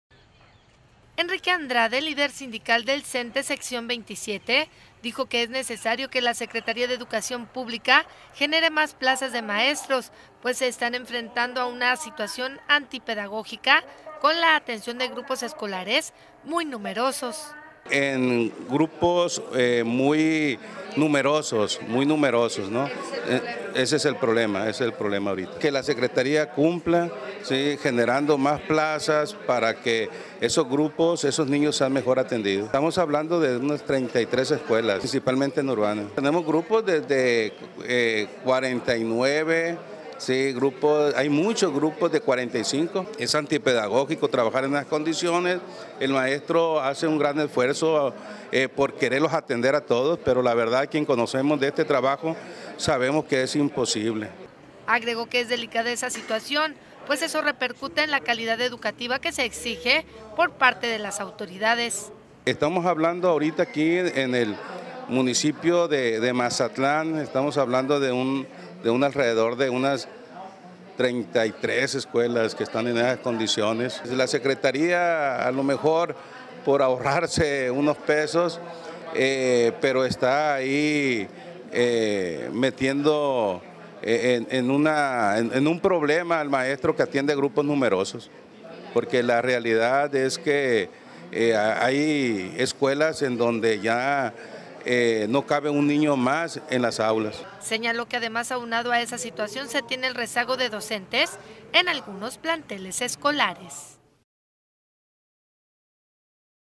Antipedagógicos grupos numerosos: SNTE 27
Enrique Andrade, Líder Sindical del Snte sección 27, dijo que es necesario que la secretaría de educación pública, genere más plazas de maestros, pues se están enfrentando a una situación antipedagógica con la atención de grupos escolares muy numerosos.

Agregó que es delicada esa situación, pues eso repercute en la calidad educativa que se exige por parte de las autoridades.

Señaló que además aunado a esa situación se tiene el rezago de docentes en algunos planteles escolares.